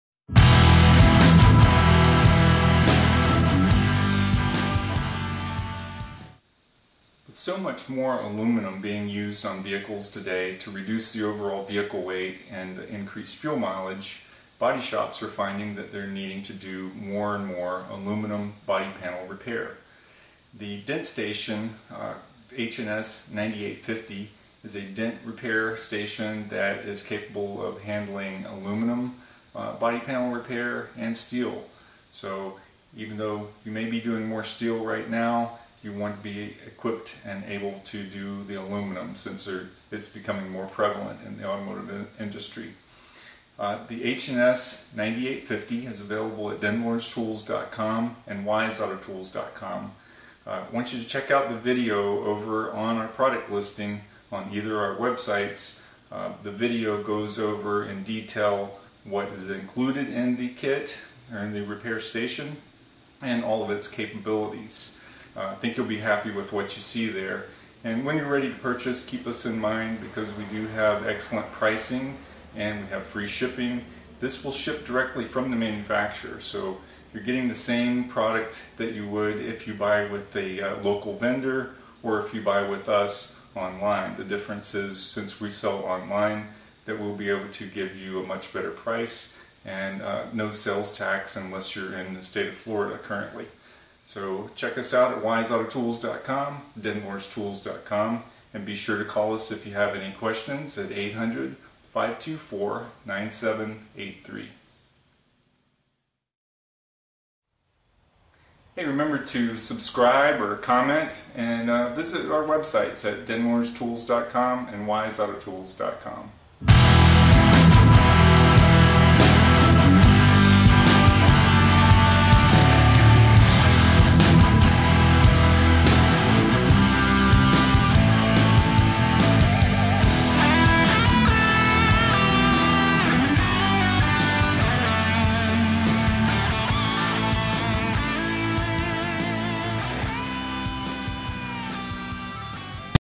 H&S Autoshot UNI-9850 Dent Repair Station for Aluminum & Steel
$6399 For aluminum and steel - paint on or paint off. Car manufacturers are using much more aluminum in vehicles today to reduce over-all weight and increase fuel mileage. One example is Ford with it’s new trucks having an aluminum cabs reducing vehicle weight by hundreds of pounds. We’ve seen it for years in engine components with aluminum heads, blocks and intakes. Suspension and steering components are used a lot too. But, now more body panels are aluminum. With aluminum body panels new tooling is needed to handle these repairs. But what if one dent repair station can handle both steel and aluminum? What if the station could also be used for paint on and paint off? If you own or work in a body shop, I think you’re going to like the H&S Autoshot 9850.